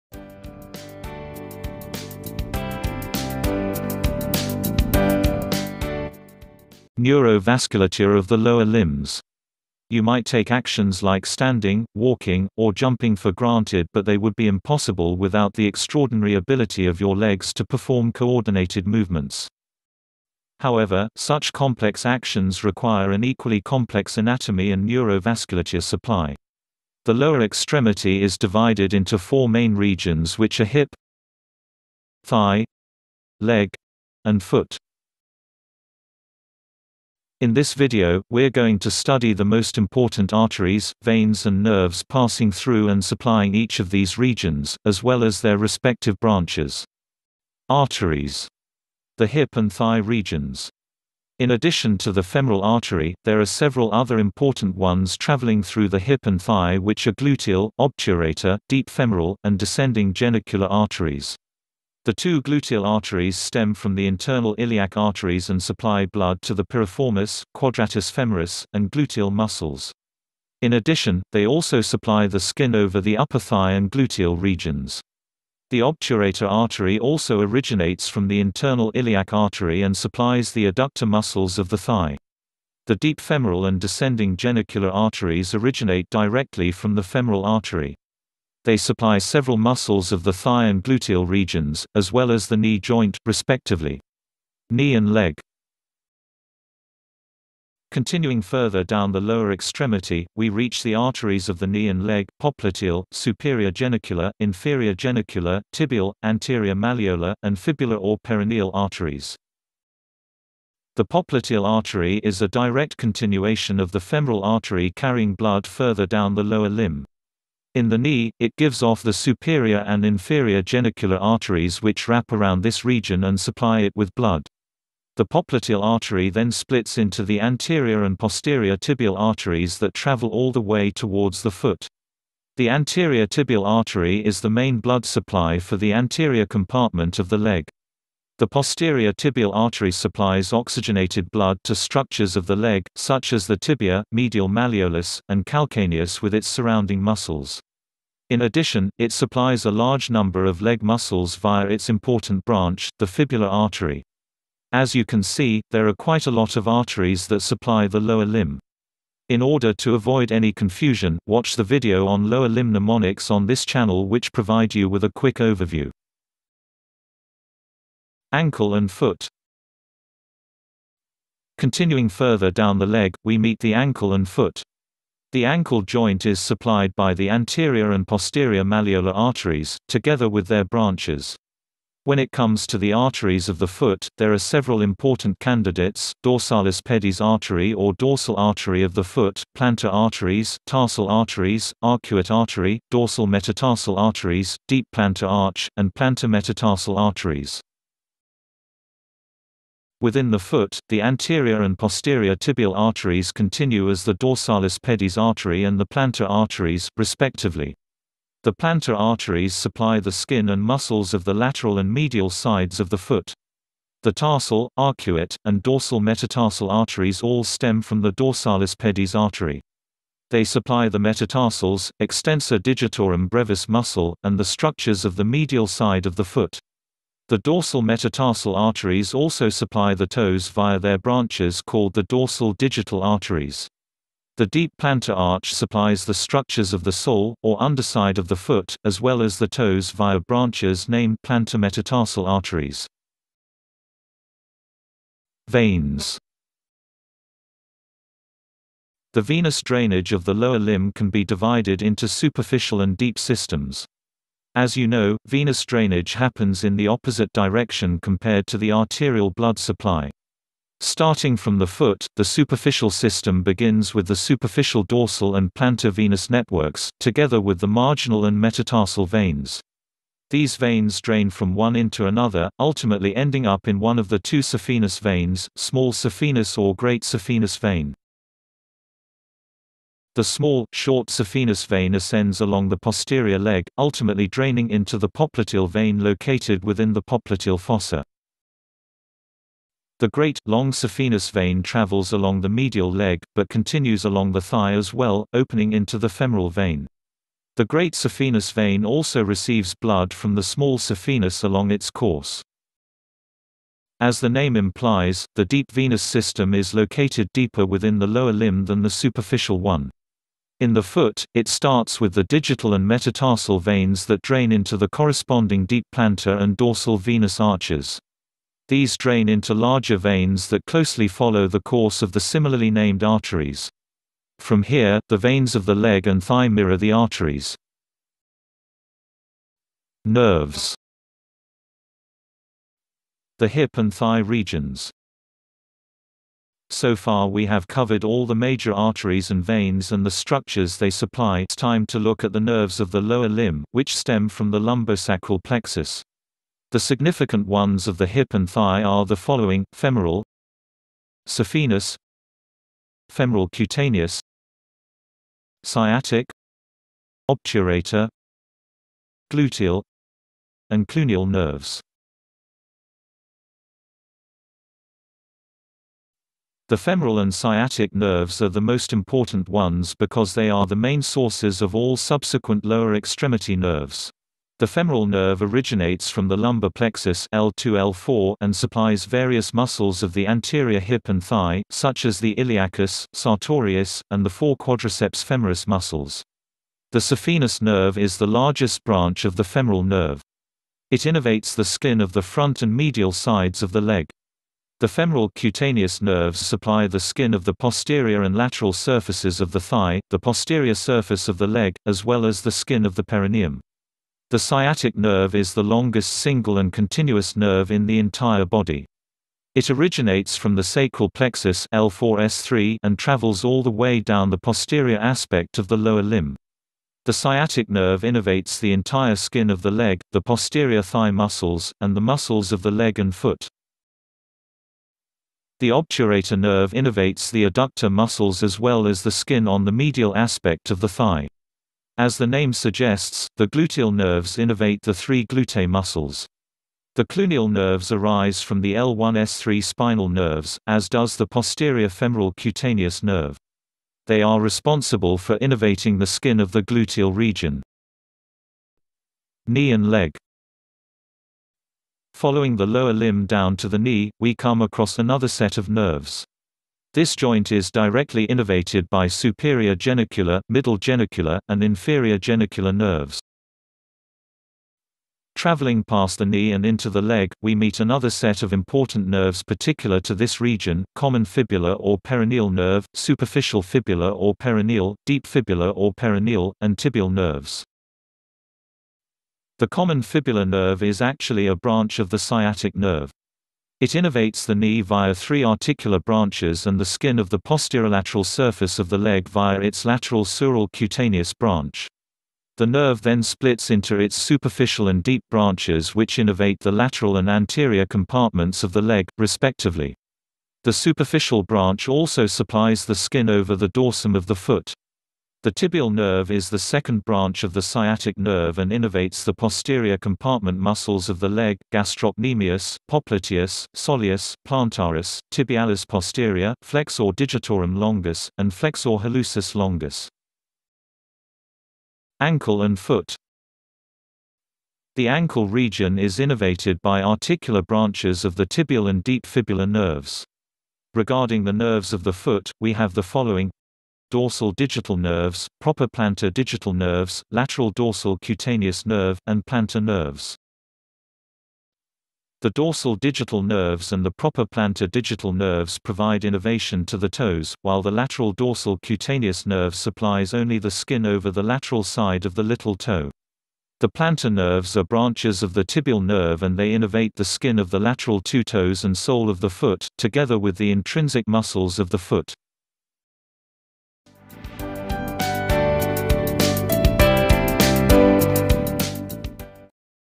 Neurovasculature of the Lower Limbs | Handwritten Tutorial.
You might take actions like standing, walking, or jumping for granted but they would be impossible without the extraordinary ability of your legs to perform coordinated movements. However, such complex actions require an equally complex anatomy and neuro-vasculature supply.
In this video, we are going to discuss the arteries, veins and nerves of each Lower limb region.